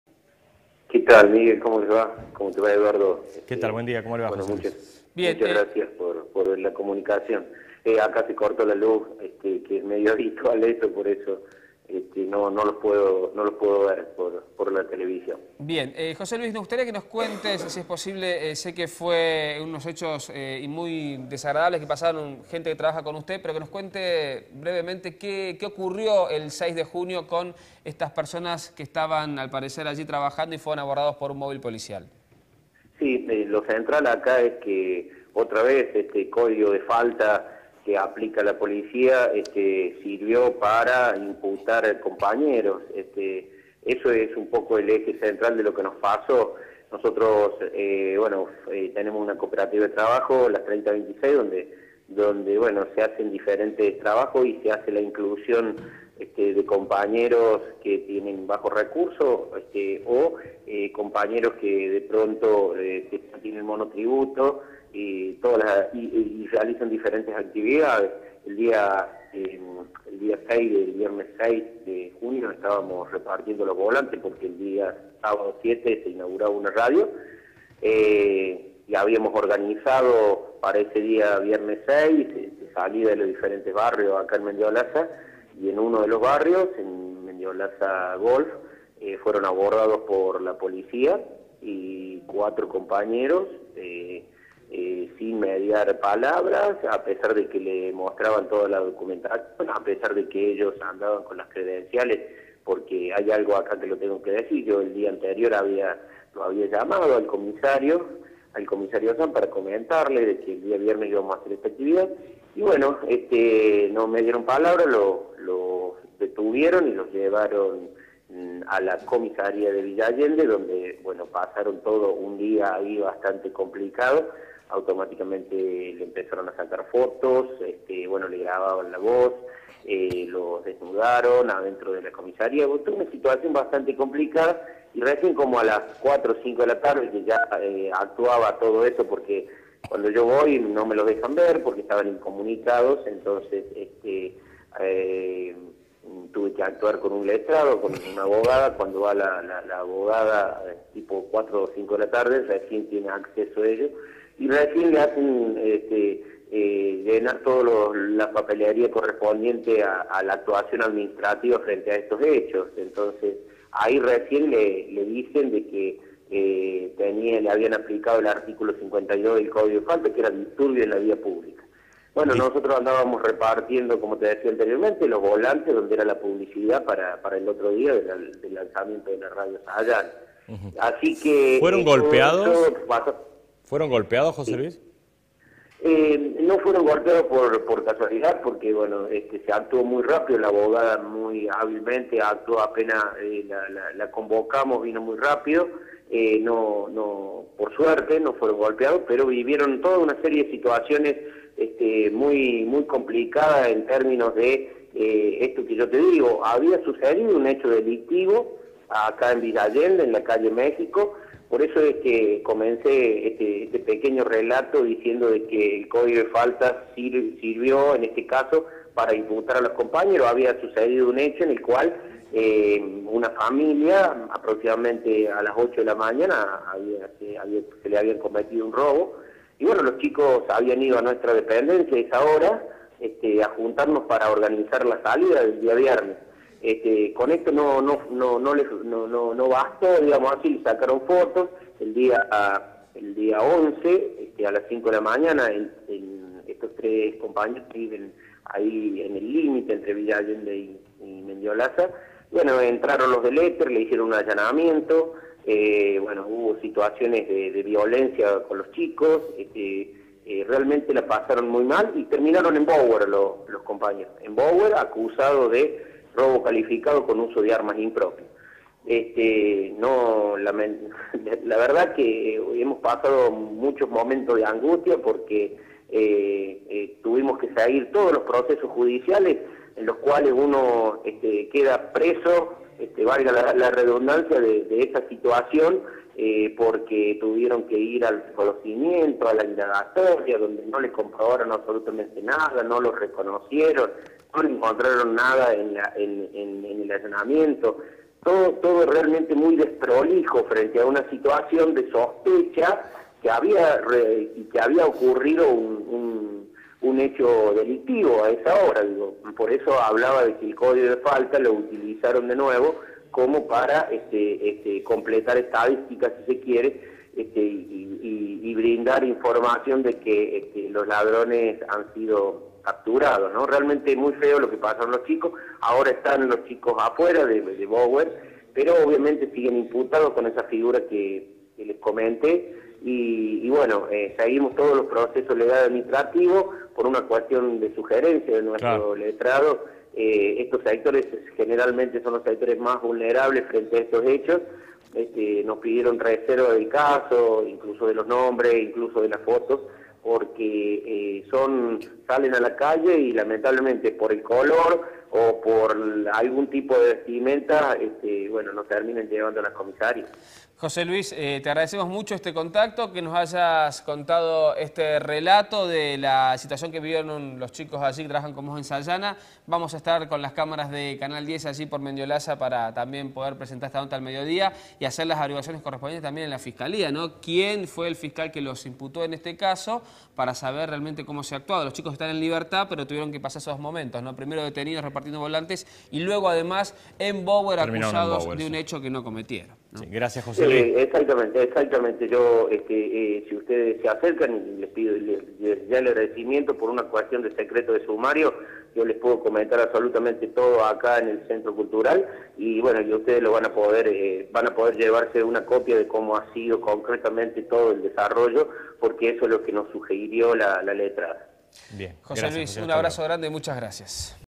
Mendiolaza: denuncian abuso policial contra tres jóvenes
Fueron arrestados mientras realizaban trabajos comunitarios. Sindicados como ladrones, los sometieron a allanamientos y requisas.